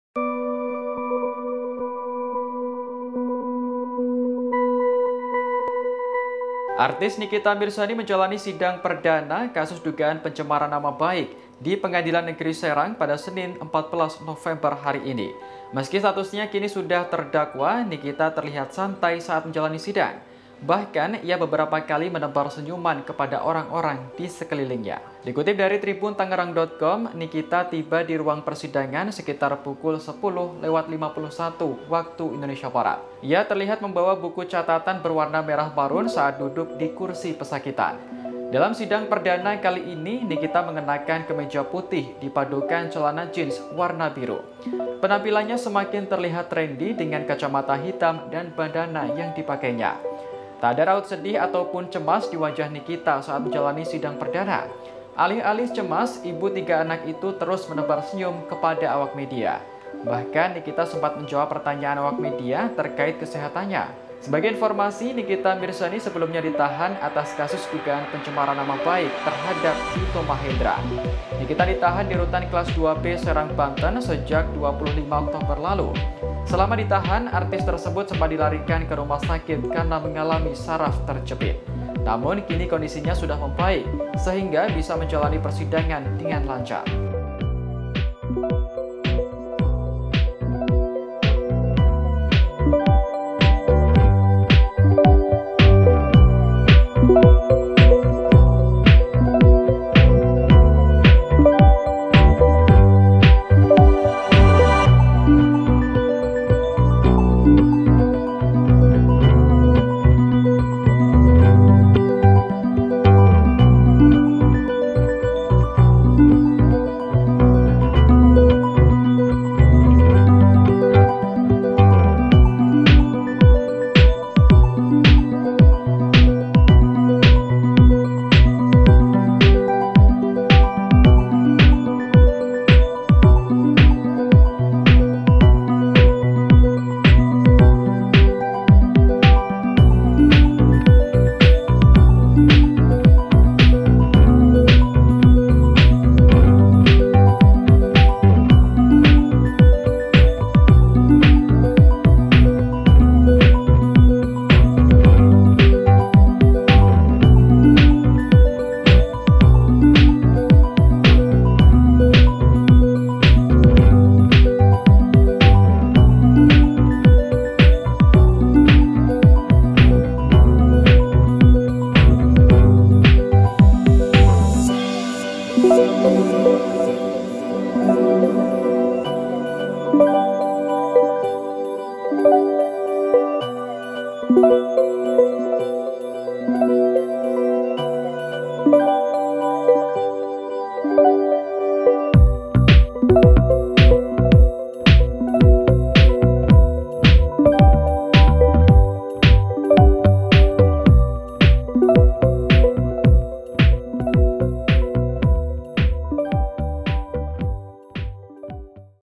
Tak Terlihat Cemas, Nikita Mirzani Malah Tebar Senyum saat Jalani Sidang Kasus Pencemaran Nama Baik
Artis Nikita Mirzani menjalani sidang perdana kasus dugaan pencemaran nama baik di Pengadilan Negeri Serang, Senin (14/11).

Meski statusnya kini sudah terdakwa, Nikita terlihat santai saat menjalani sidang.

Bahkan, ia beberapa kali menebar senyuman kepada orang-orang di sekelilingnya.